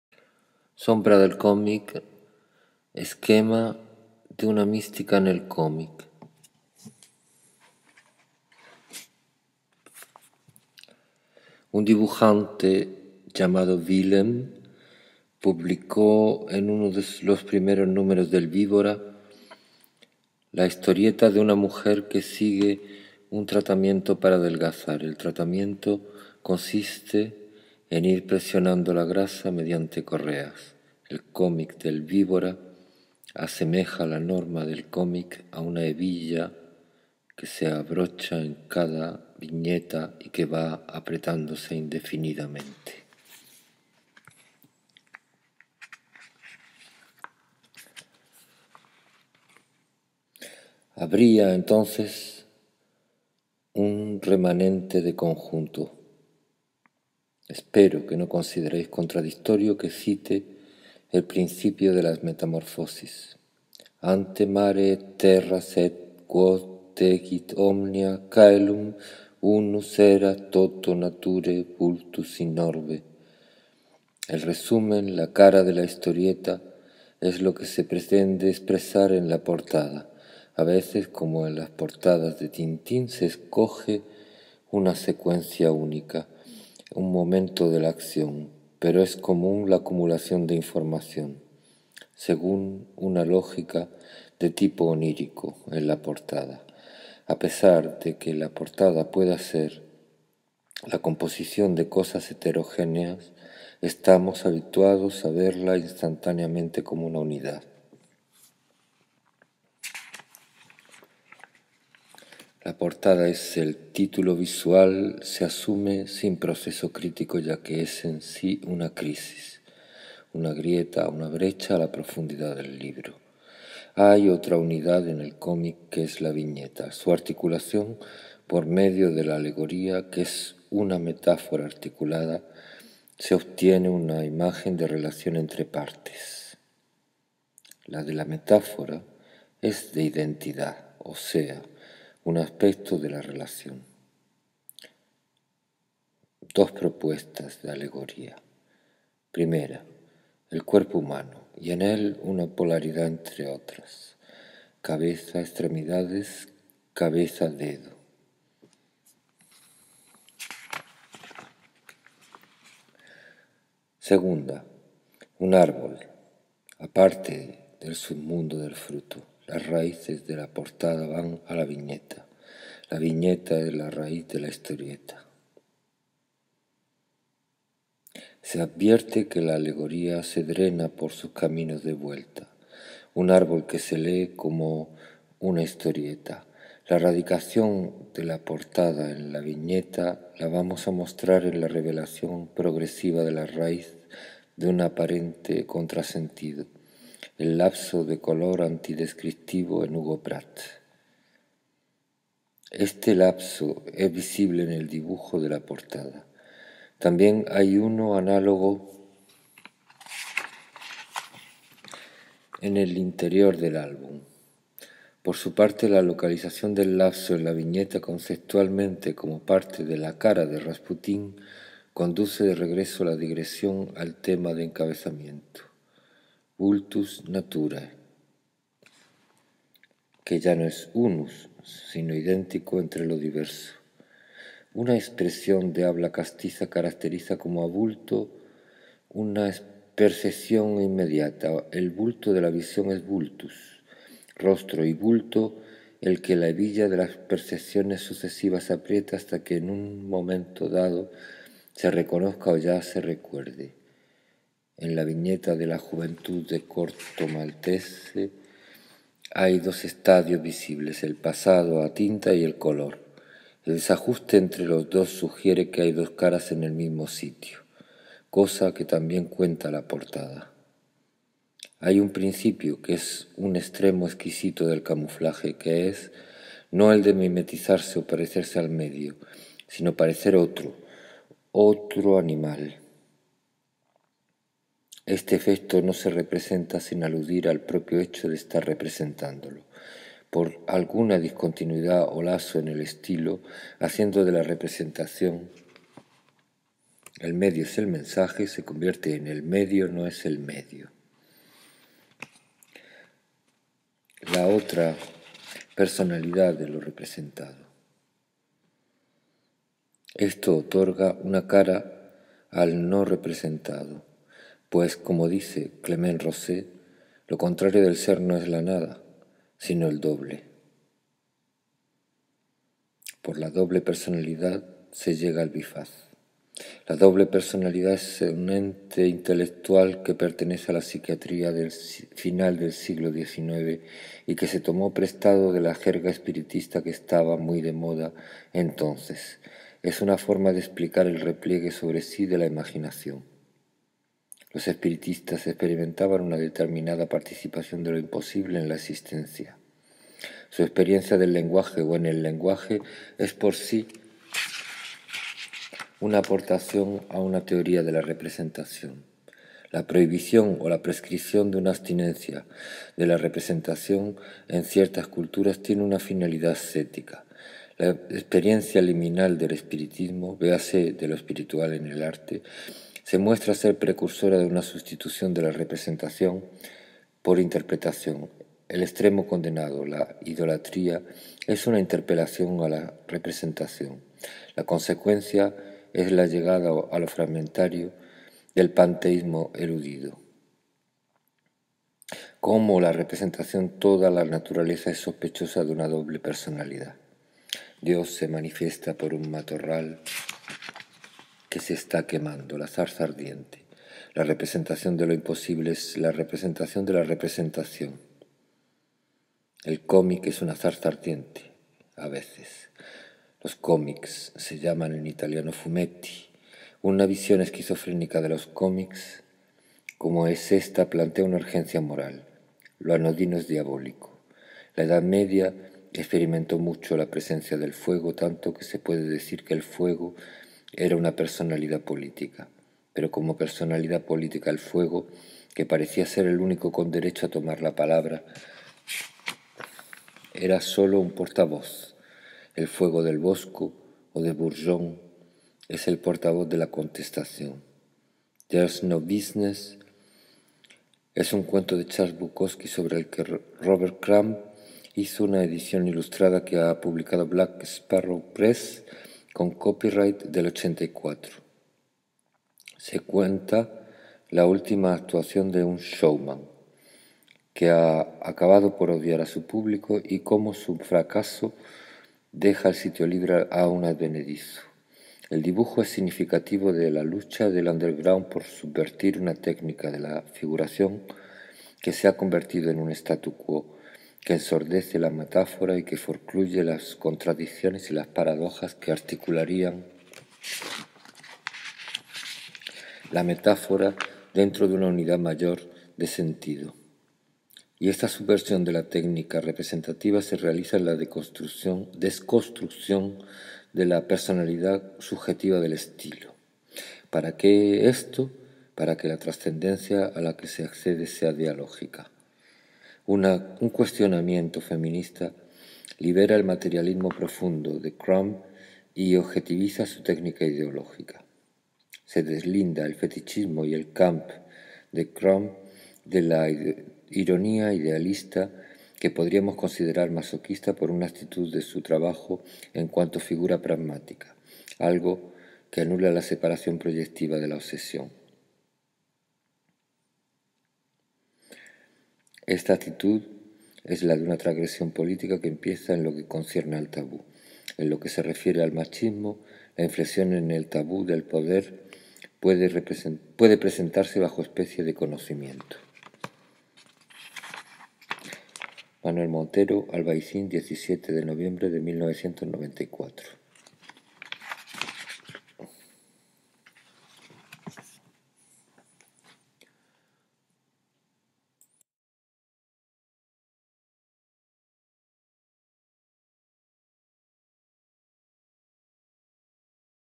sombra del cómic 720p
Con ilustraciones de las Tijuana Bible, Rambla, Segunda sonrisa, Tonatustra, Jarabe de palo, Weirdo, Raw... de fecha amplia (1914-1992)
texto de la conferencia dictada en Casa de Porras, Albaicin, en 1994
editada en 2001 por la Facultad de Bellas Artes
filmado y grabado en Paris en 2021
MM